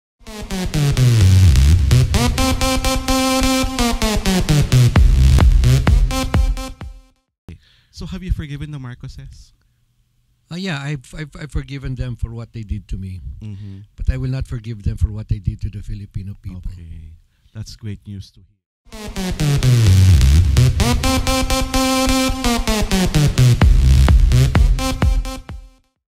Serge Osmeña III sa mga Marcos: Napatawad ko na sila, ngunit ‘di sa mga ginawa nila sa Pilipino
Nang tanungin kung napatawad na ba niya ang mga Marcos, hindi nagdalawang-isip si Serge Osmeña III sa isasagot niya at sinabing napatawad na niya ang mga ito ngunit hindi pa niya ito napapatawad sa nagawa nito sa mga Pilipino.